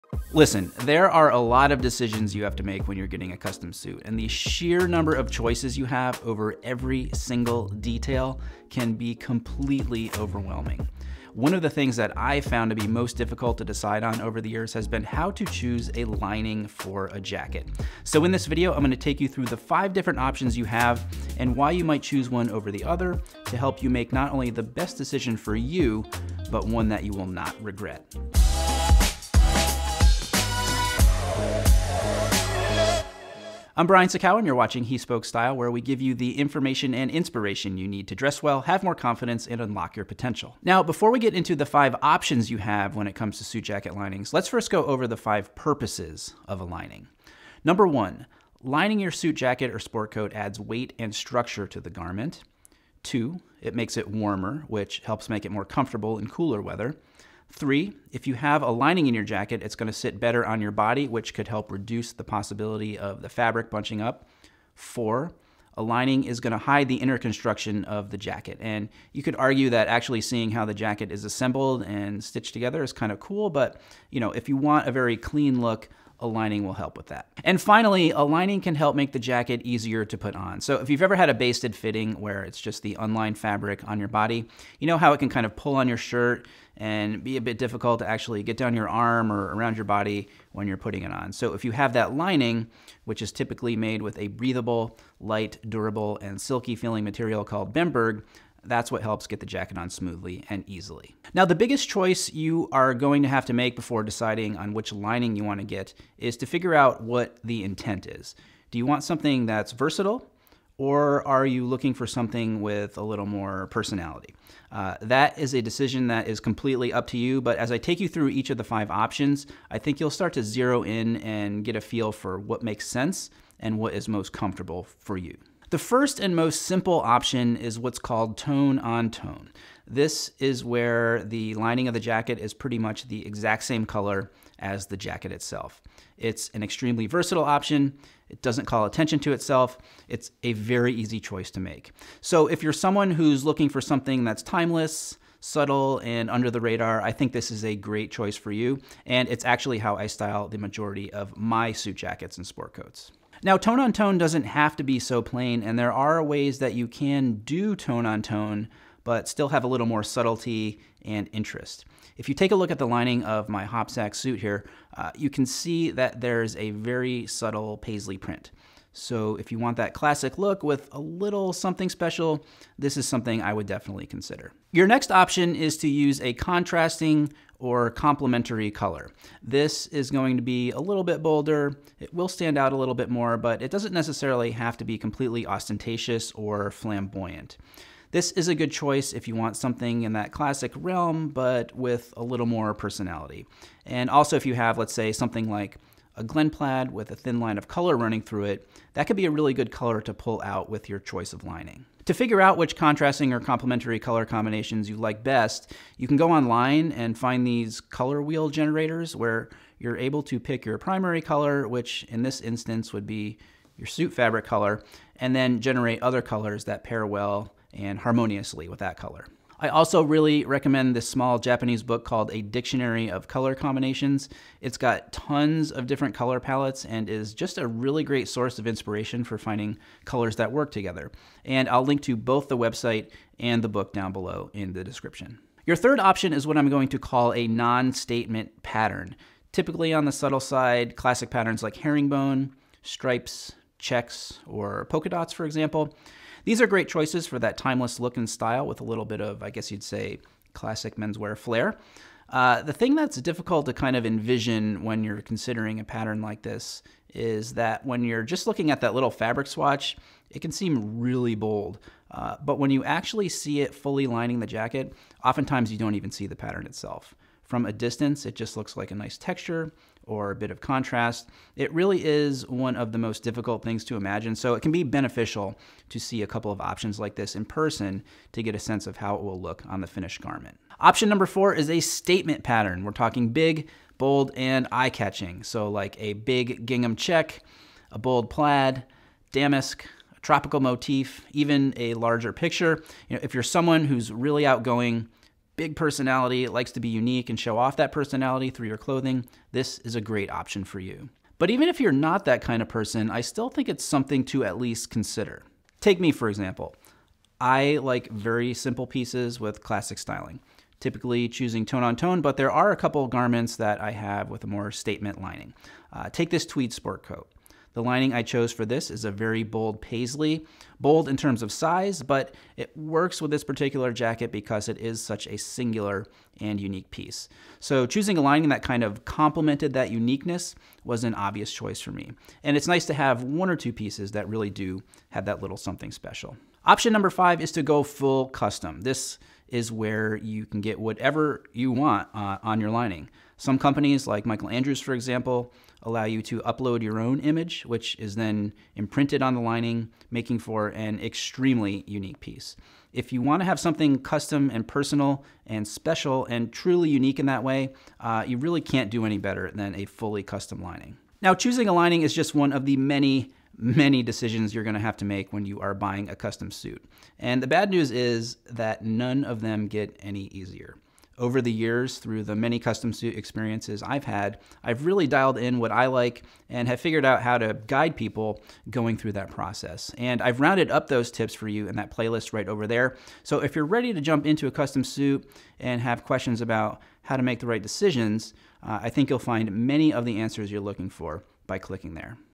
How To Choose A Suit Jacket Lining For Made To Measure Suits
One of the best things about made to measure suits and bespoke suits is the amount of control you have over every single detail. Yet, for the novice, that amount of choice can often be overwhelming. In this video, I'm focusing on one particular detail of suit jacket design--the suit jacket lining--breaking down the five options you have as well as how to choose the suit lining that is right for you and your personality.

0:00 - How To Choose A Suit Lining
0:44 - Purposes Of A Suit Lining
2:05 - Tone On Tone
2:44 - Complementary/Contrasting Color
3:55 - Non-Statement Pattern
4:40 - Statement Pattern
5:36 - Full Custom